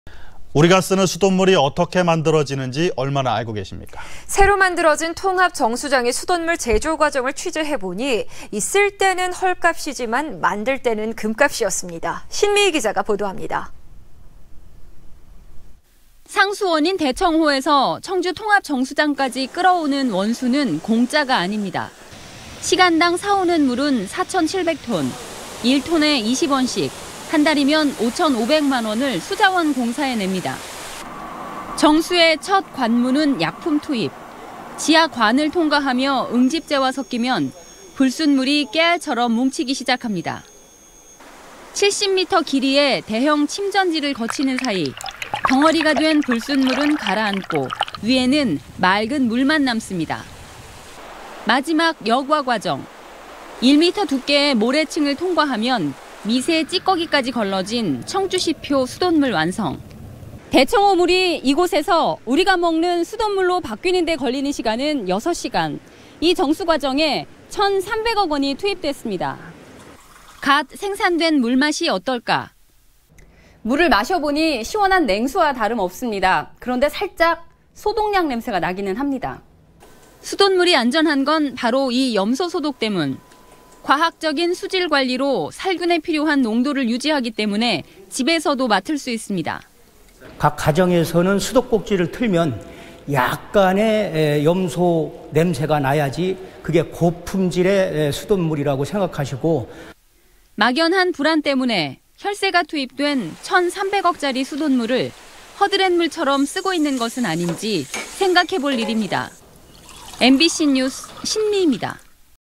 [청주MBC뉴스] 천3백억짜리 수돗물
◀앵커▶
 우리가 쓰는 청주의 수돗물이 어떻게
만들어 지는 지 얼마나 알고 계십니까?
 새로 만들어진 통합 정수장의 수돗물
제조 전 과정을 취재했는 데  쓸 때는
헐값이지만 만들때는 금값이 투입됩니다.
신미이 기자가 보도합니다.

           ◀ＥＮＤ▶

 상수원인 대청호에서
청주 통합정수장까지 끌어오는 원수는
공짜가 아닙니다.

시간당 사오는 물은 4천 7백톤.
1톤에 20원씩, 한달이면 5천 5백만원을
수자원공사에 냅니다.

 정수의 첫 관문은 약품 투입
지하 관을 통과하며  응집제와 섞이면
불순물이 깨알처럼 뭉치기 시작합니다.

 70미터 길이의 대형 침전지를 거치는 사이
덩어리가 된 불순물은 가라 앉고
위에는 맑은 물만 남습니다.

 마지막 여과 과정,
 1미터 두께의 모레층을 통과하면
미세 찌꺼기까지 걸러진 청주시표 수돗물 완성.

 S/U 신미이 "대청호 물이 우리가 먹는
수돗물로 바뀌는데 걸리는 시간은 불과 6시간,
이 정수과정에 1300억원이 투입됐습니다.  "

갓 생산된 물맛이 어떨까?

 S/U "직접 마셔보겠습니다.
시원한 냉수맛입니다. 그런데 살짝 소독 냄새가 나기는 합니다."

 수돗물이 안전한 건 바로 이 염소 소독때문,
과학적인 수질 관리로 살균에 필요한 농도를 유지하기 때문에 집에서도 맡을 수 있습니다.

           ◀ＩＮＴ▶
 이중훈/청주시 상수도사업본부장
" 소독약 냄세가 나야 안전한 물 "

 막연한 불안 때문에
혈세가 투입된 천3백억짜리 수돗물을
허드렛물처럼 쓰고 있는 것은 아닌지
생각해 볼 일입니다.
 MBC뉴스 신미입니다.